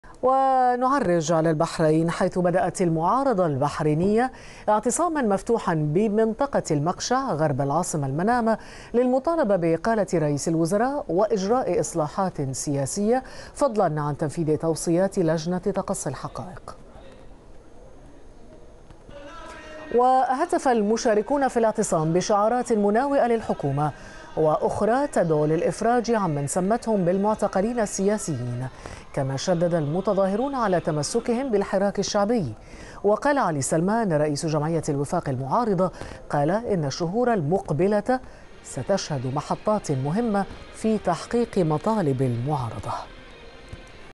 الجزيرة:المعارضة البحرينية تبدأ اعتصاماً مفتوحاً-5-2-2012
جمعية الوفاق الوطني الإسلامية
المركز الإعلامي
تقرير قناة الجزيرة الفضائية حول الاعتصام المفتوح الذي دعت إليه قوى المعارضة البحرينية في ساحة الحرية بمنطقة المقشع غربي المنامة للمطالبة بإصلاحات سياسية من بينها إقالة الحكومة الحالية.
5-2-2012